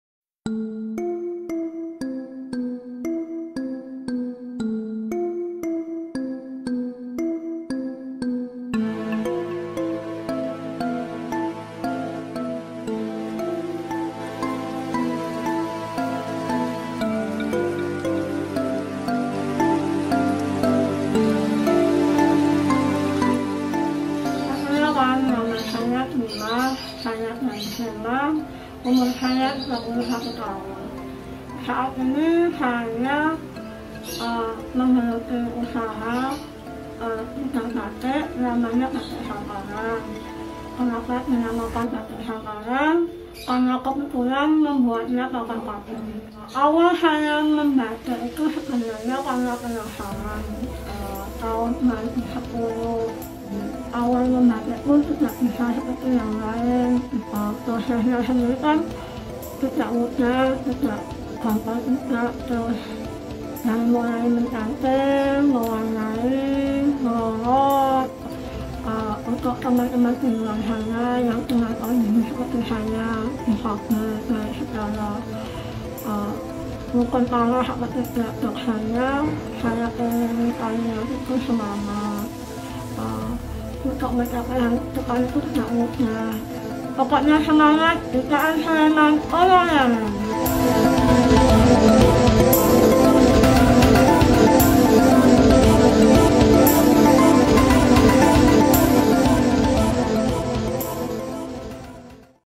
Kisah Mbak Trimah Batik Samparan, Membatik menggunakan batik kaki
keterbatasan bukan penghalang untuk terus mempersembahkan sebuah karya.
adalah Mbak Trimah, ibu dari satu orang anak yang mempunyai semangat untuk terus belajar membatik, menggunakan kaki.